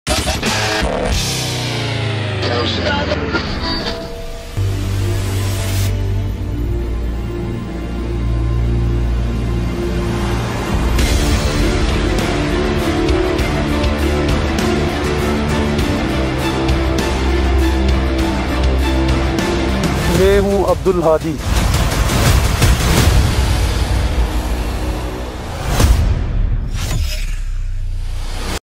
Polo Wars | Amenarchy | Abdul Hady | Shandur Knights
PoloWars : Abdul Hady - GB Police

These knights of Shandur have never before been seen as the true heroes they are. Their talents and feats need to be recognised as they partake in an ancient sport few are given the chance at and even fewer have the bravery and skill to play.